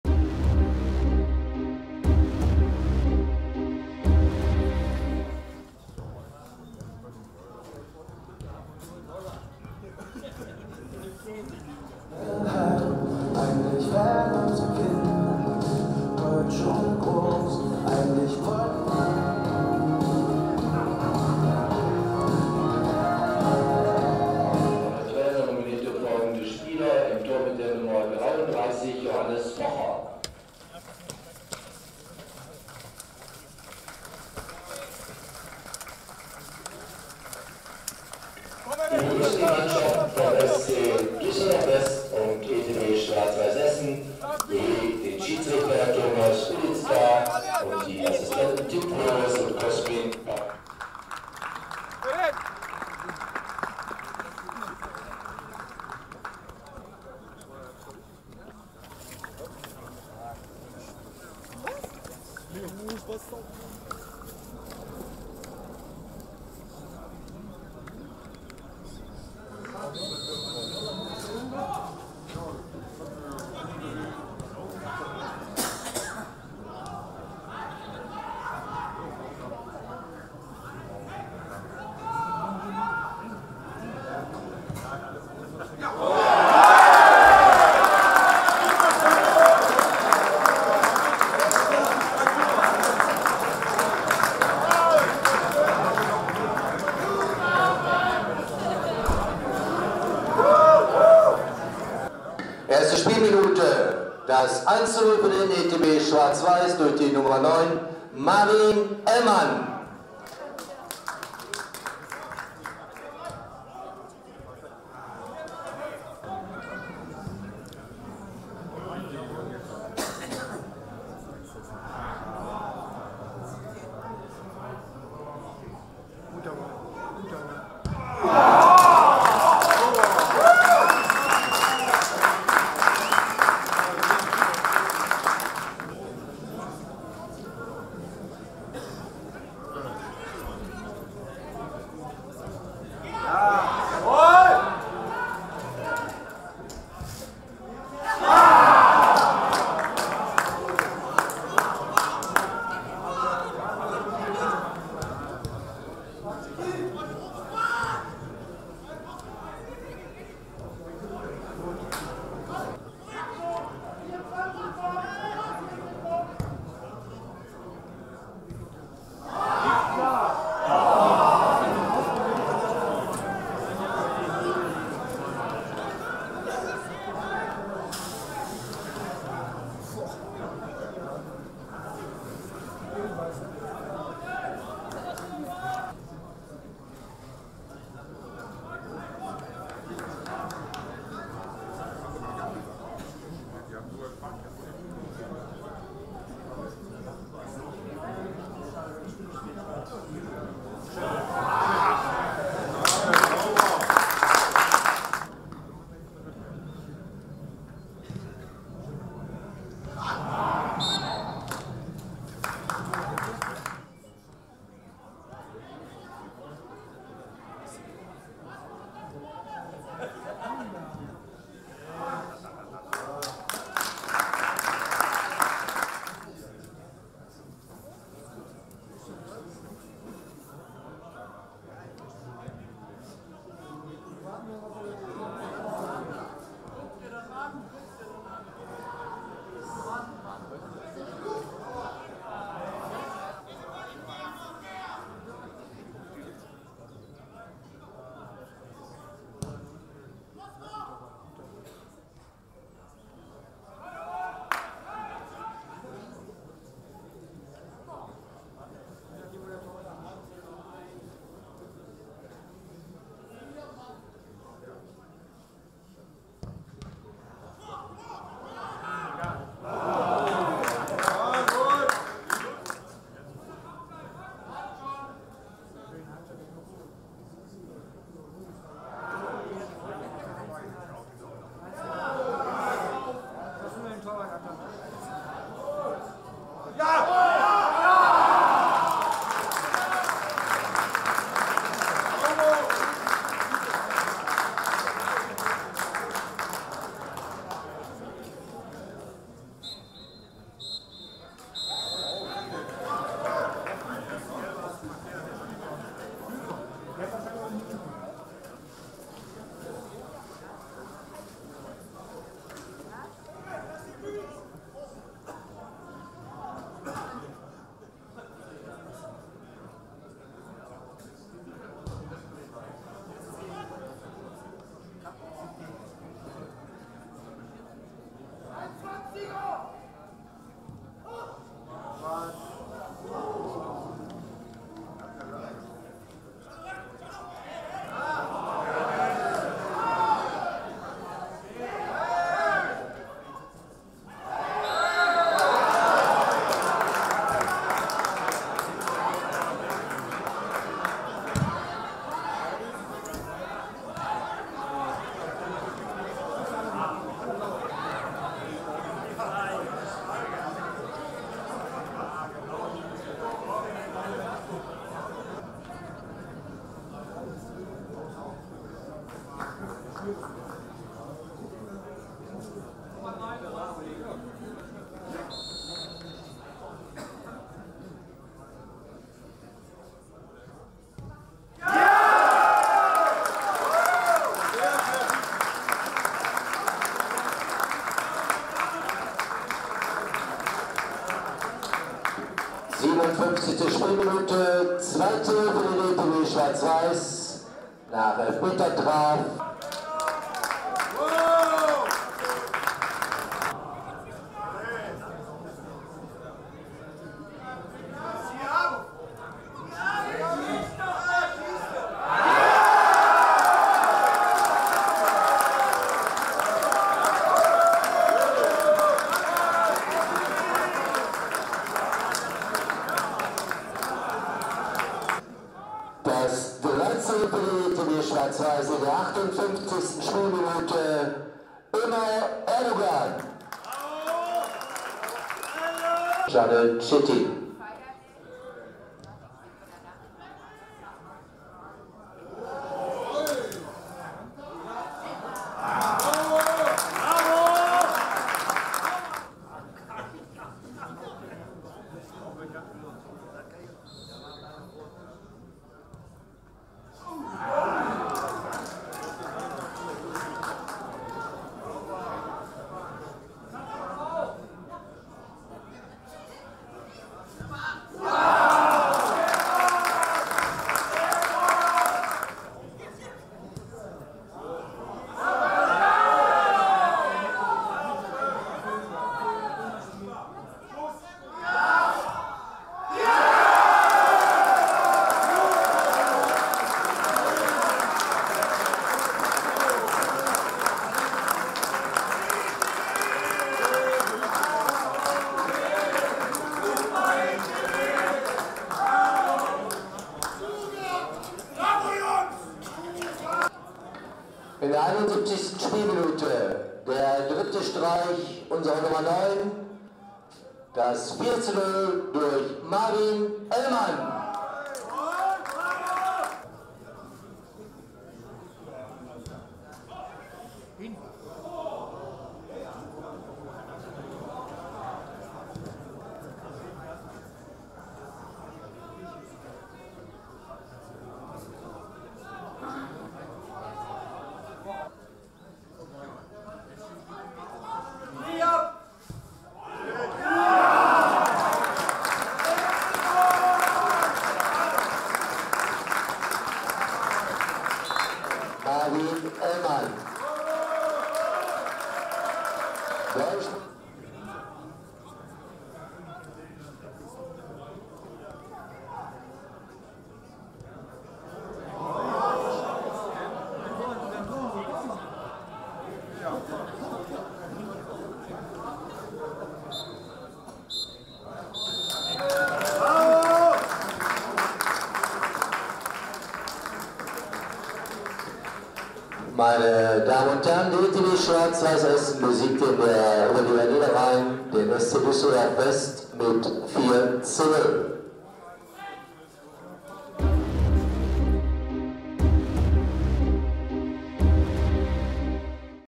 FV N'rh Oberliga Saison 2017 18 SP26 ETB SW Essen vs SC Düsseldorf West 29 3 2018
Oberliga FV-N'rh. Saison 2017/18  26.Spieltag
ETB SW Essen vs. vs. SC Düsseldorf - West
Stadion Uhlenkrug, Am Uhlenkrug  (Essen)